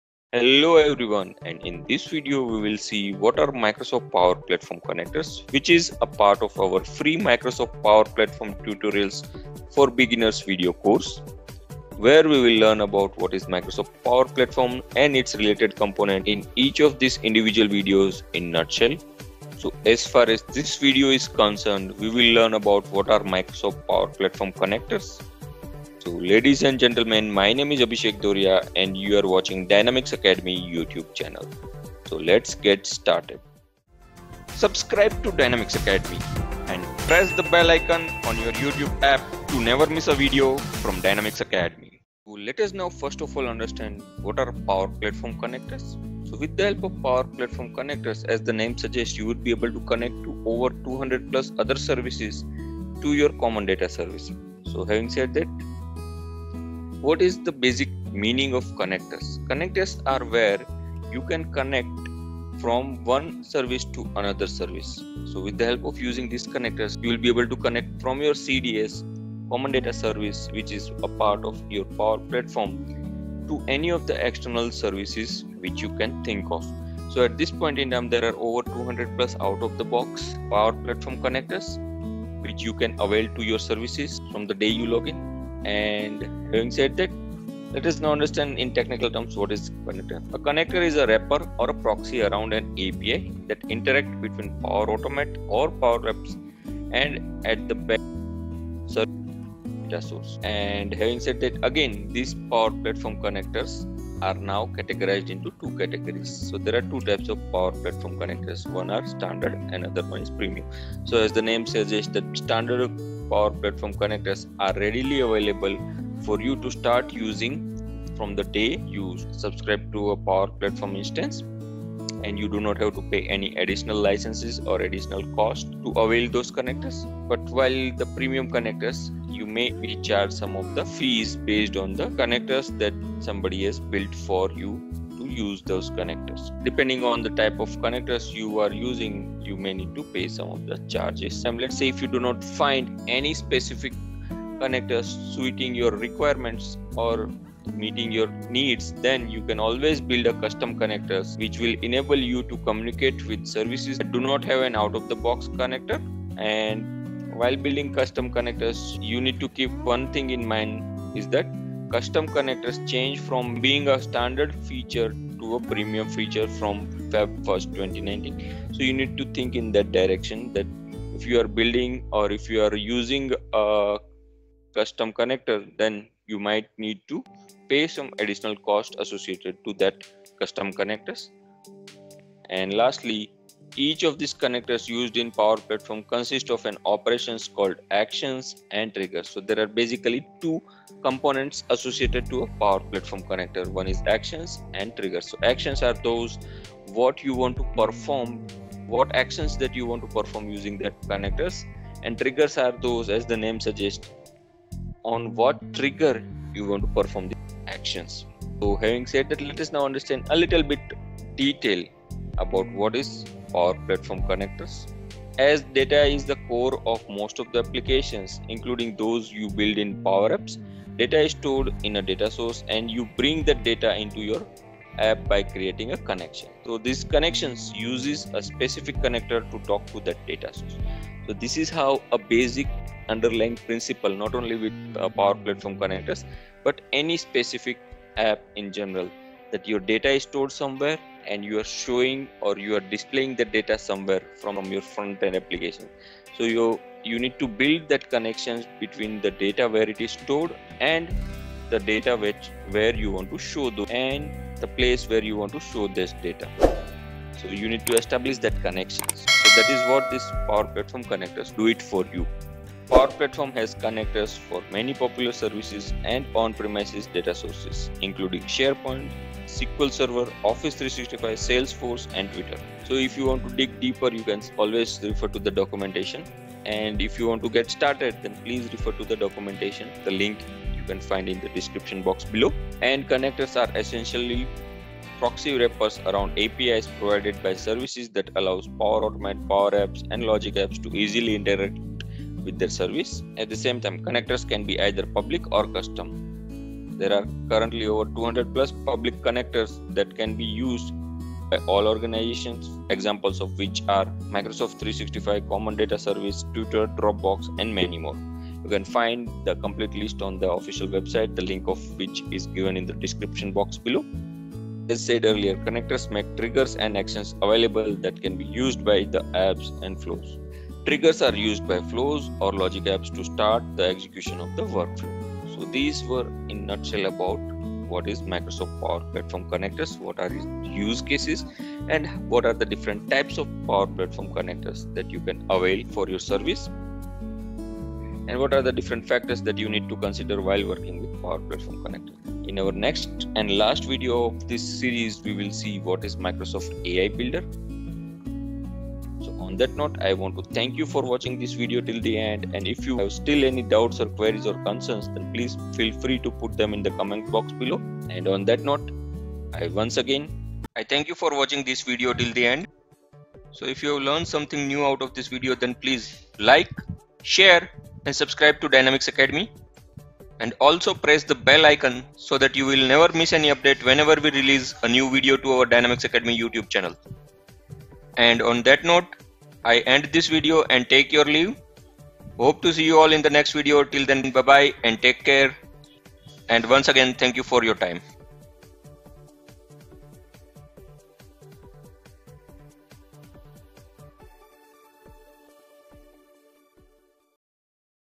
What are Power Platform Connectors or power apps connectors ?
What are Power Platform Connectors?

Below are two courses that you can take to start your journey of becoming a Microsoft Dynamics 365 Developer.

Data is at the core of most apps, including those you build in Power Apps. Data is stored in a data source, and you bring that data into your app by creating a connection. The connection uses a specific connector to talk to the data source.
Power Platform has connectors for many popular services and on-premises data sources, including SharePoint, SQL Server, Office 365, Salesforce, and Twitter. To get started adding data to a canvas app, see Add a data connection in Power Apps.
Connectors are essentially proxy wrappers around the APIs provided by services that allow Power Automate, Power Apps, and Logic Apps to easily interact with the service.
Connectors can be either public or custom. There are currently over 200+ public connectors that can be used by all organizations. Examples of public connectors are Microsoft 365, Common Data Service, Twitter, Dropbox and more.
Connectors make triggers and actions available that can be used by the apps and flows. Triggers are used by flow or Logic Apps to start the execution of the workflow.
Actions are used by apps and flows to perform a defined set of actions during execution.

Recommended Playlist – What's New in Microsoft Dynamics 365? New Features in V9.0